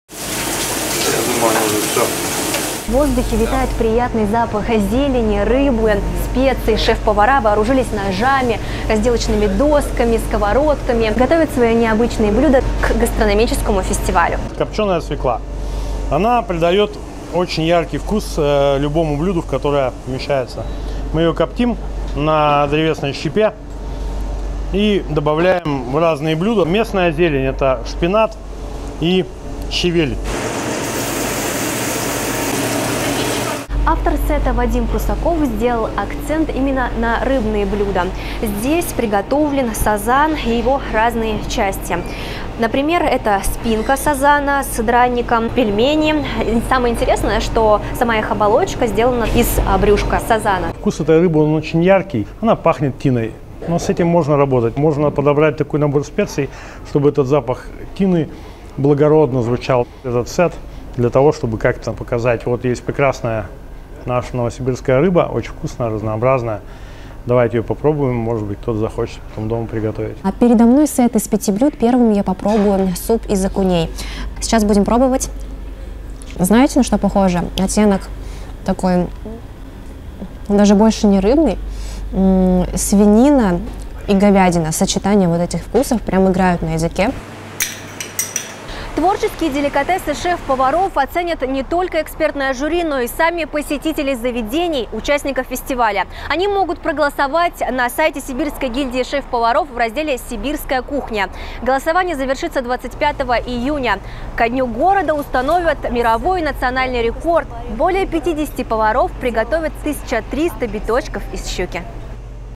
Блюда с благородным запахом тины приготовили сибирские шеф-повара
Блюда из пеляди, окуня и сазана представили участники гастрономического фестиваля «Сибирская кухня». Шеф-повара из семи заведений города разработали авторские сеты из местных продуктов. Вадим Прусаков приготовил блюда из сазана.